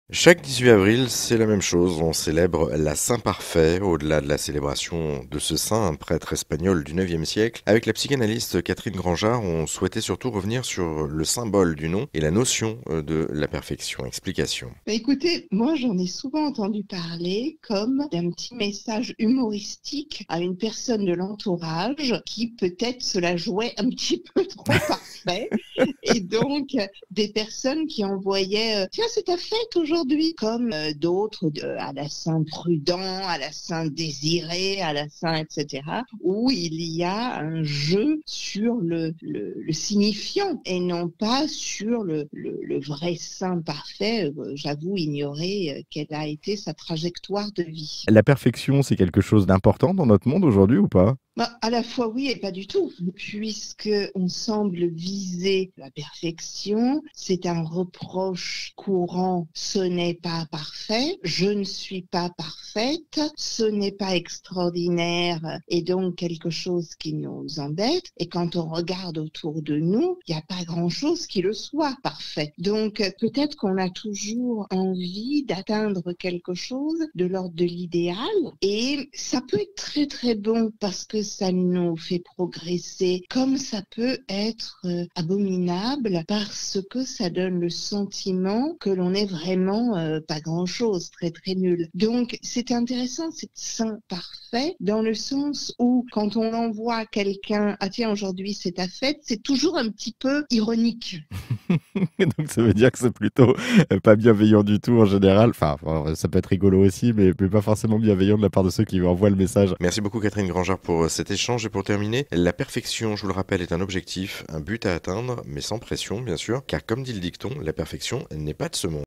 Saint Parfait - La perfection en question
Souhaiter être parfait, ça fait du bien ?
pas forcément ! Ce sujet mérite bien que l'on s'y attarde, qu'on réfléchisse...
Les journées internationales permettent de s'interroger sur le thème de la dite journée.

Pour aller plus loin :
Ce contenu audio a été diffusé sur AirZen Radio. Maintenant disponible en podcast sur airzen.fr  et toutes les plateformes de streaming.
interview par Jerôme Pasanau, JOURNALISTE.

Et bien sûr on peut retrouver les liens et les détails sur mon blog :

Air Zen - Catherine Grangeard